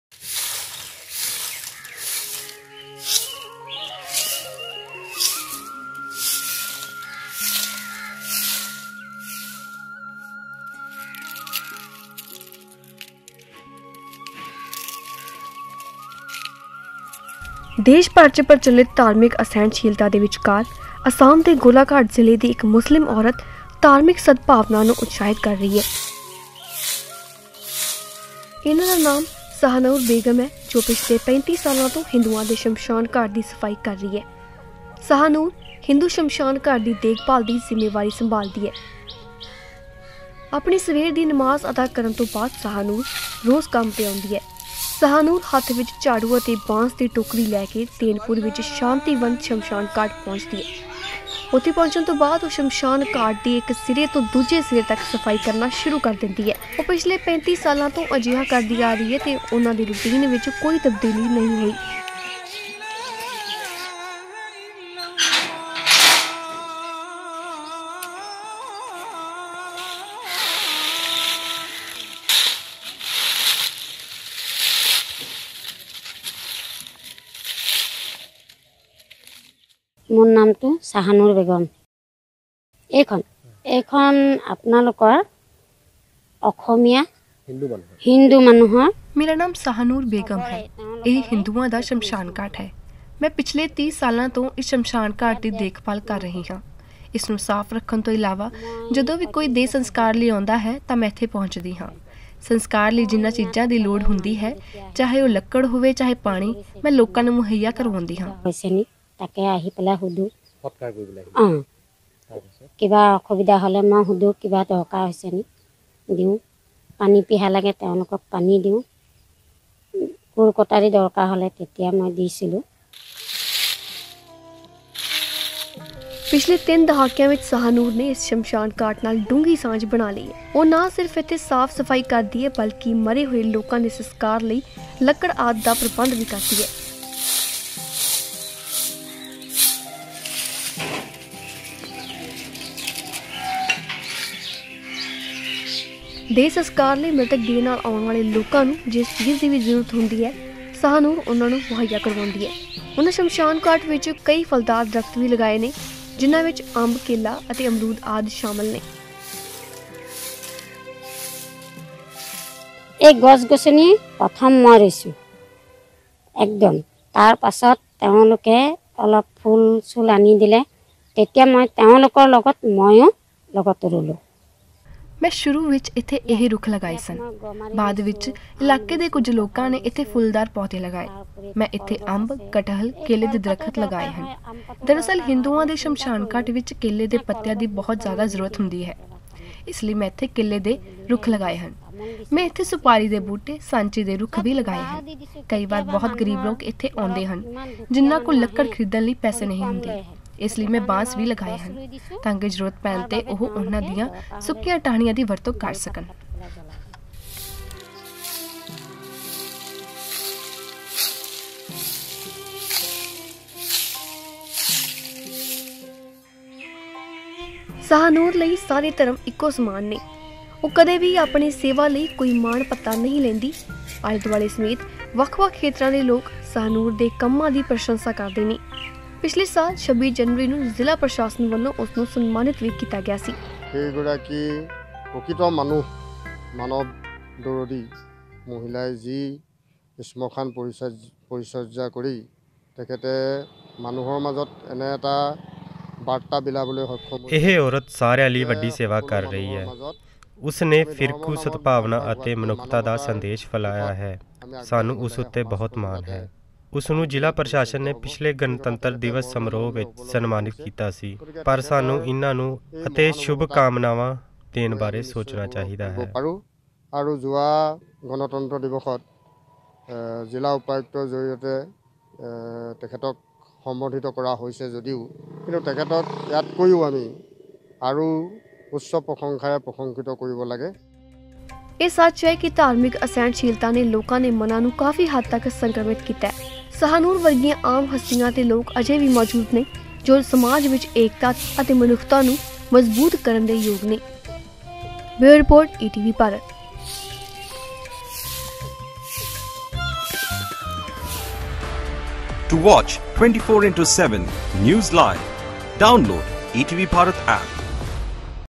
ਹਿੰਦੂ ਸ਼ਮਸ਼ਾਨ ਦੀ ਦੇਖਭਾਲ ਕਰਨ ਵਾਲੀ ਮੁਸਲਿਮ ਸਹਾਰਨੂਰ ਬੇਗਮ
ਦੇਸ਼ ਭਰ 'ਚ ਪ੍ਰਚਲਿਤ ਧਾਰਮਿਕ ਅਸਹਿਣਸ਼ੀਲਤਾ ਦੇ ਵਿਚਕਾਰ, ਅਸਾਮ ਦੇ ਗੋਲਾਘਾਟ ਜ਼ਿਲ੍ਹੇ ਦੀ ਇੱਕ ਮੁਸਲਿਮ ਔਰਤ ਧਾਰਮਿਕ ਸਦਭਾਵਨਾ ਨੂੰ ਉਤਸ਼ਾਹਤ ਕਰ ਰਹੀ ਹੈ। ਇਨ੍ਹਾ ਦਾ ਨਾਮ ਸਹਿਨੂਰ ਬੇਗਮ ਹੈ ਜੋ ਪਿਛਲੇ 35 ਸਾਲਾਂ ਤੋਂ ਹਿੰਦੂਆਂ ਦੇ ਸ਼ਮਸ਼ਾਨਘਾਟ ਦੀ ਸਫਾਈ ਕਰ ਰਹੀ ਹੈ। ਸਹਾਨੂਰ ਹਿੰਦੂ ਸ਼ਮਸ਼ਾਨਘਾਟ ਦੀ ਦੇਖਭਾਲ ਦੀ ਜ਼ਿੰਮੇਵਾਰੀ ਸੰਭਾਲਦੀ ਹੈ।